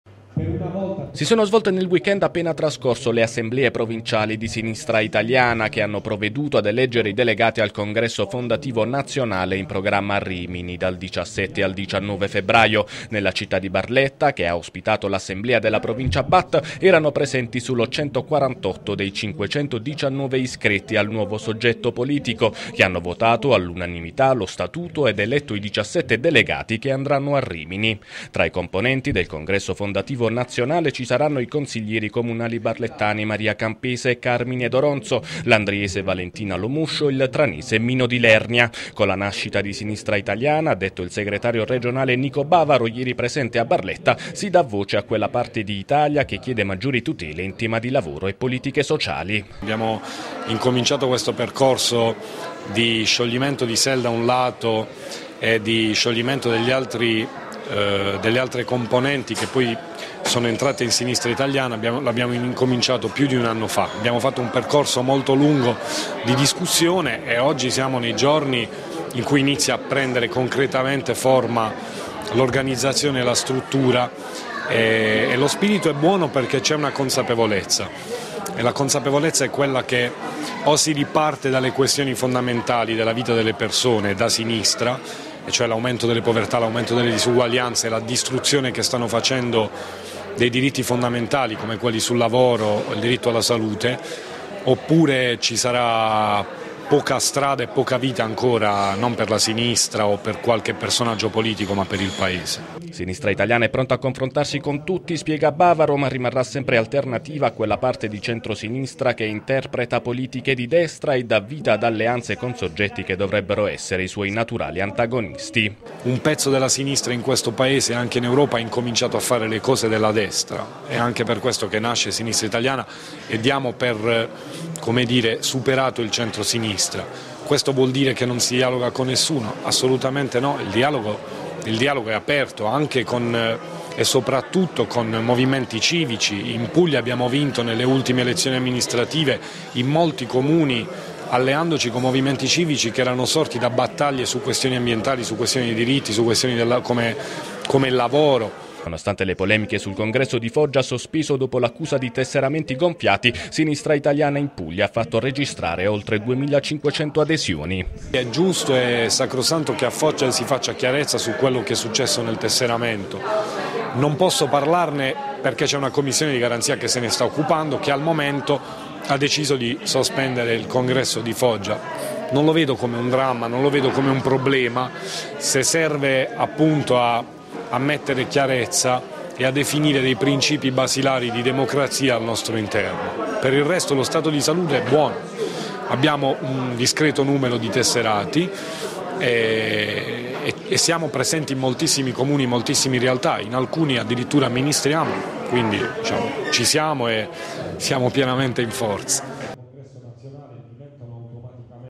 Barletta | Assemblea provinciale di Sinistra Italiana: votati i delegati per il congresso fondativo
Si è svolta ieri a Barletta, l'assemblea provinciale di Sinistra Italiana, che ha votato lo statuto ed eletto i 17 delegati che parteciperanno al congresso fondativo nazionale, in programma a Rimini, dal 17 al 19 febbraio.